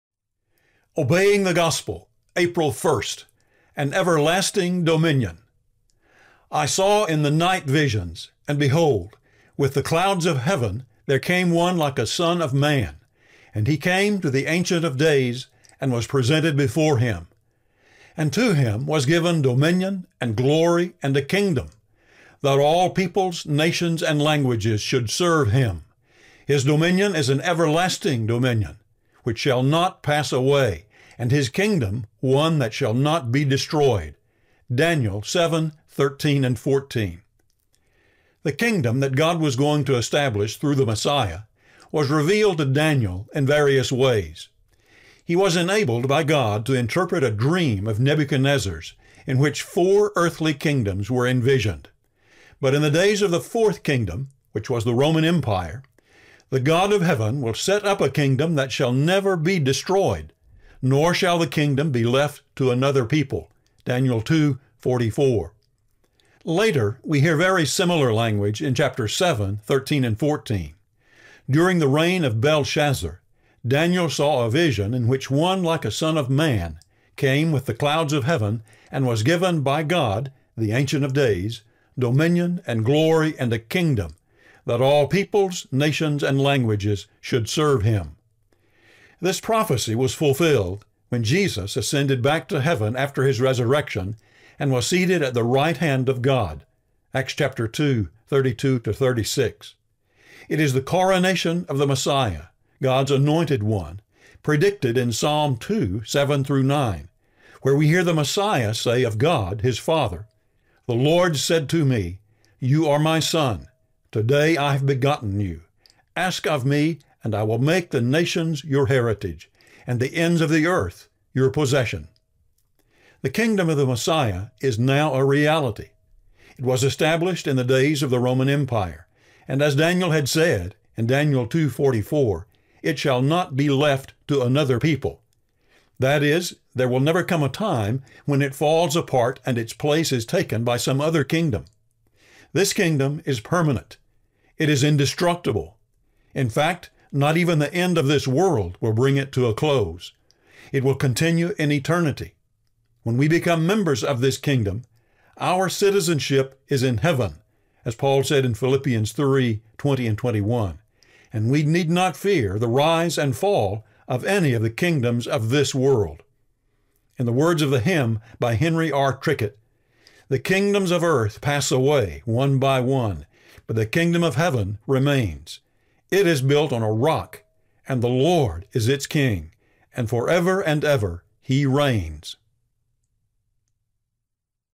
An Everlasting Dominion (April 1)
"I saw in the night visions, and behold, with the clouds of heaven there came one like a son of man, and he came to the Ancient of Days and was presented before him. And to him was given dominion and glory and a kingdom, that all peoples, nations, and languages should serve him; his dominion is an everlasting dominion, which shall not pass away, and his kingdom one that shall not be destroyed" (Daniel 7:13,14).

THE KINGDOM THAT GOD WAS GOING TO ESTABLISH THROUGH THE MESSIAH WAS REVEALED TO DANIEL IN VARIOUS WAYS. He was enabled by God to interpret a dream of Nebuchadnezzar’s in which four earthly kingdoms were envisioned, but in the days of the fourth kingdom (which was the Roman Empire), “the God of heaven will set up a kingdom that shall never be destroyed, nor shall the kingdom be left to another people” (Daniel 2:44).

Later, we hear very similar language in 7:13,14. During the reign of Belshazzar, Daniel saw a vision in which “one like a son of man” came with the clouds of heaven and was given by God, the Ancient of Days, “dominion and glory and a kingdom, that all peoples, nations, and languages should serve him.”

This prophecy was fulfilled when Jesus ascended back to heaven after His resurrection and was seated at the right hand of God (Acts 2:32-36). It is the coronation of the Messiah, God’s Anointed One, predicted in Psalm 2:7-9, where we hear the Messiah say of God, His Father: “The Lord said to me, ‘You are my Son; today I have begotten you. Ask of me, and I will make the nations your heritage, and the ends of the earth your possession.’”

The kingdom of the Messiah is now a reality. It was established in the days of the Roman Empire, and as Daniel had said in 2:44, it shall not “be left to another people.” That is, there will never come a time when it falls apart and its place is taken by some other kingdom. This kingdom is permanent. It is indestructible. In fact, not even the end of this world will bring it to a close; it will continue in eternity. When we become members of this kingdom, our “citizenship is in heaven” (Philippians 3:20,21) — and we need not fear the rise and fall of any of the kingdoms of this world.

The kingdoms of earth pass away one by one,
But the kingdom of heaven remains;
It is built on a rock, and the Lord is its king,
And forever and ever He reigns.
(Henry R. Trickett)